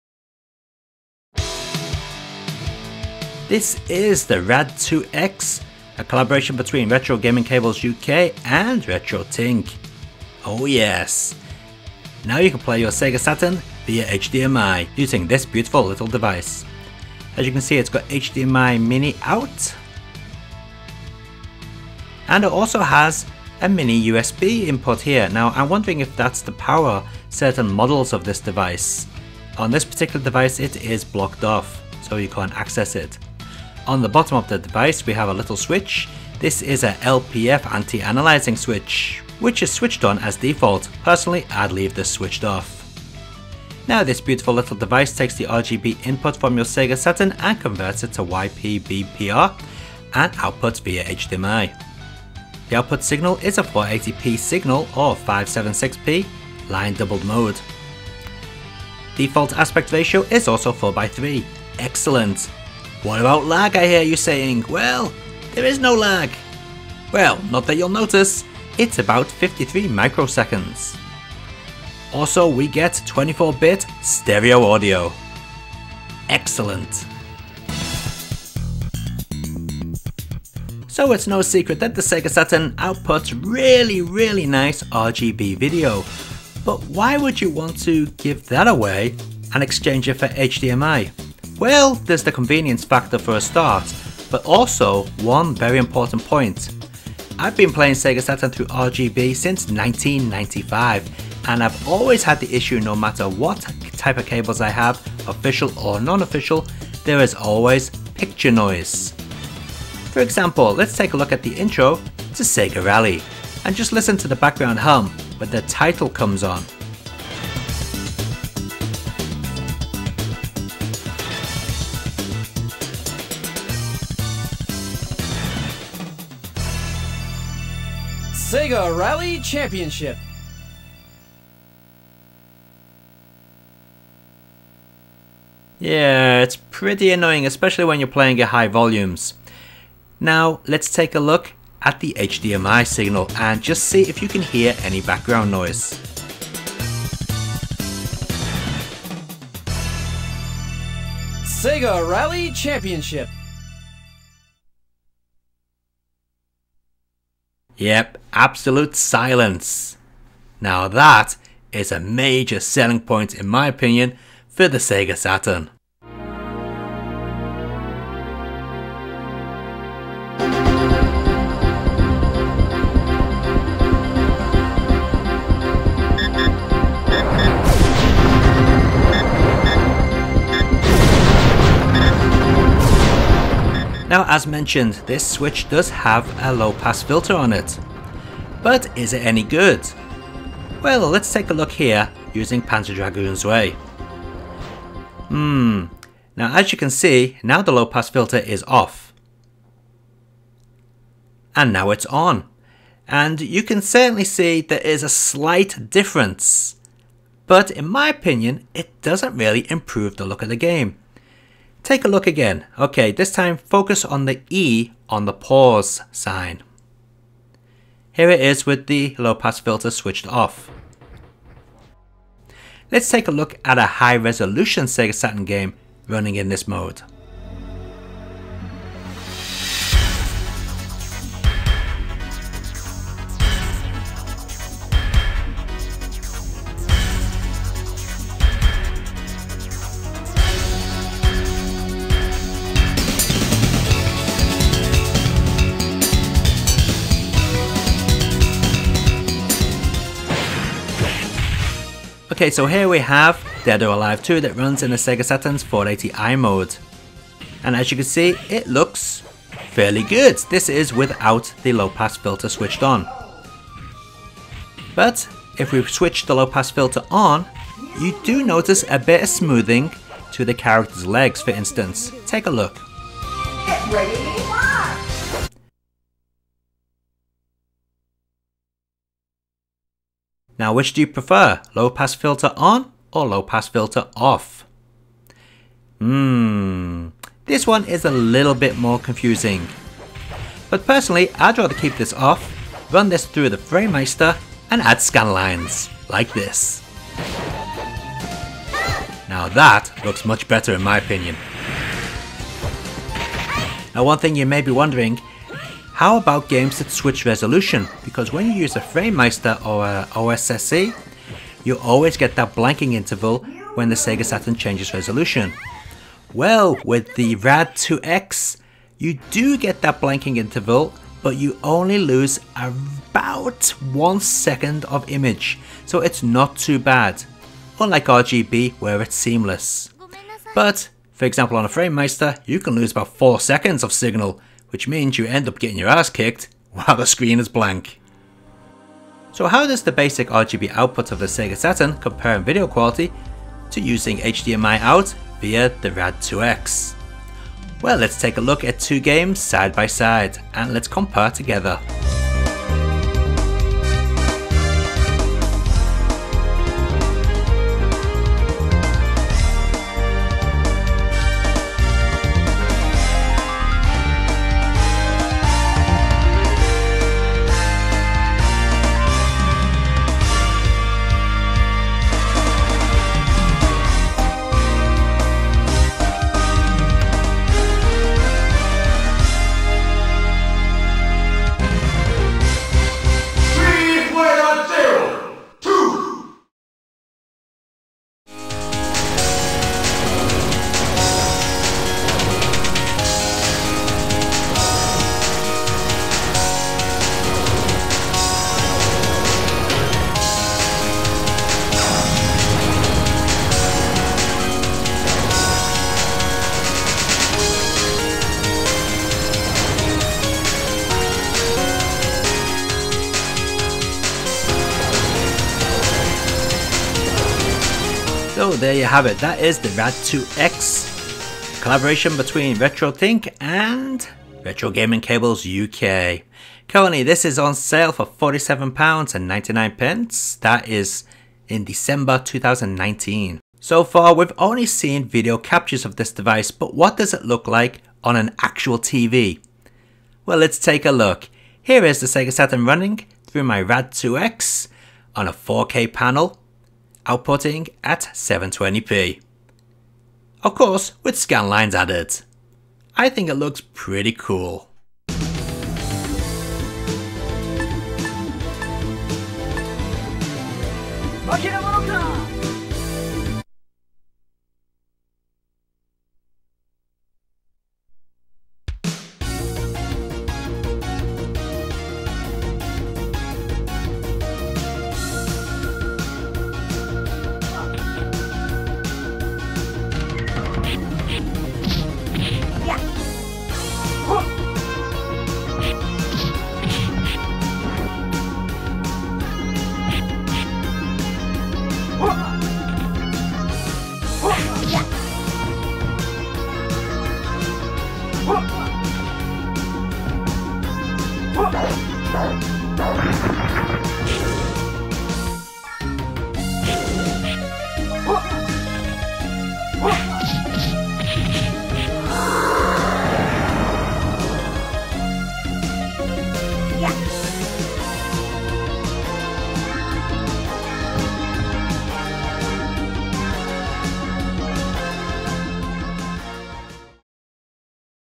RAD2X - HDMI for your Sega Saturn
Lack the ability to use RGB?
Stuck using crappy composite video or the slightly better S-Video?
Then maybe you need the RAD2X HDMI solution for your Sega Saturn.

Full review and side by side comparison to RGB.  I bought my own unit from Retro Gaming Cables UK.